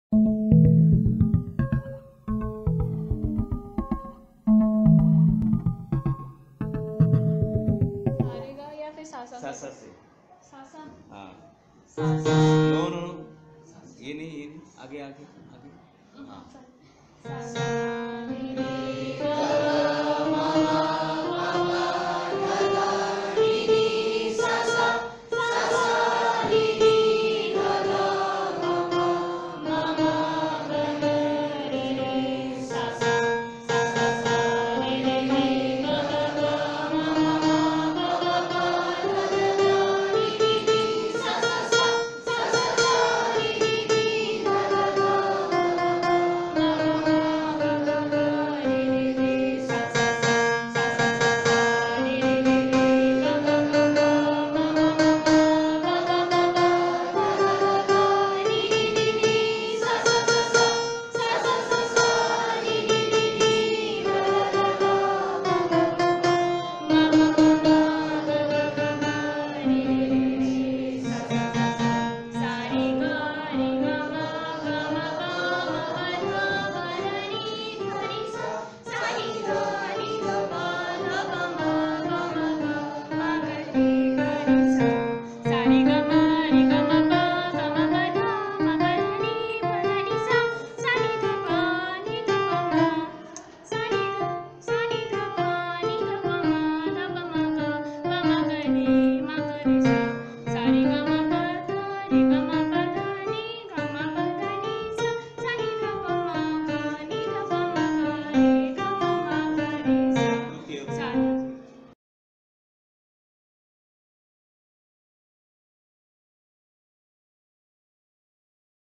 Video flipped Music decoded
A wonderful first day performance of Students of APS Dinjan !
Std IX